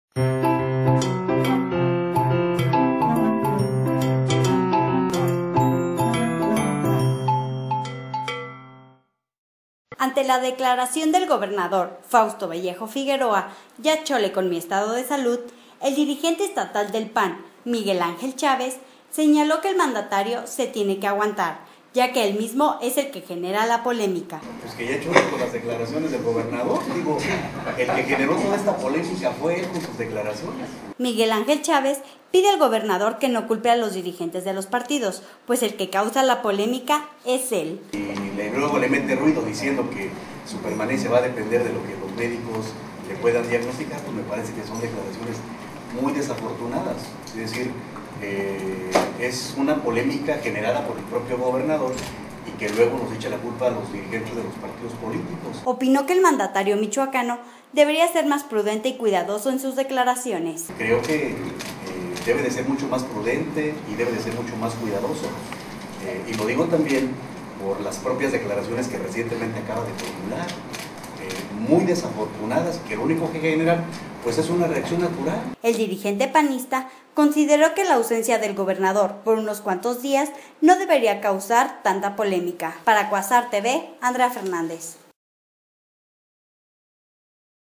Fausto es quien genera polémica sobre su salud, PAN
Ante la declaración del gobernadorm Fausto Vallejo "ya chole con mi estado de salud", el dirigente estatal del PAN, Miguel Ángel Chávez señaló que el mandatario se tiene que aguantar, ya que él mismo es el que genera polémica...